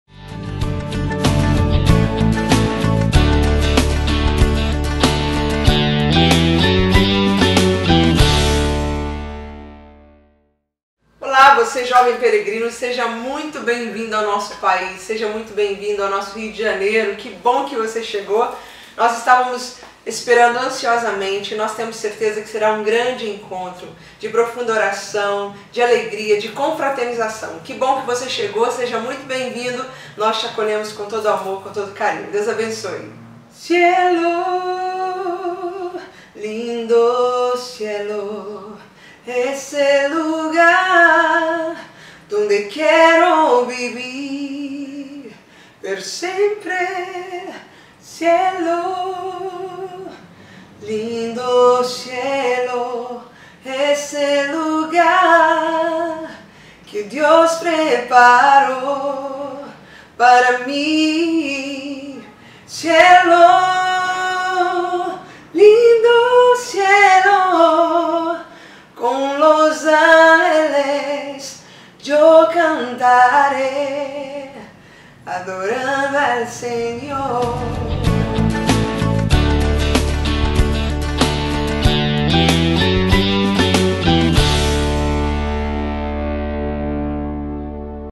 Bem-vindo à JMJ Rio2013 - Adriana Arydes
Adriana Arydes e Paulinas-COMEP dão as boas-vindas a todos os peregrinos que participarão da Jornada Mundial da Juventude Rio2013!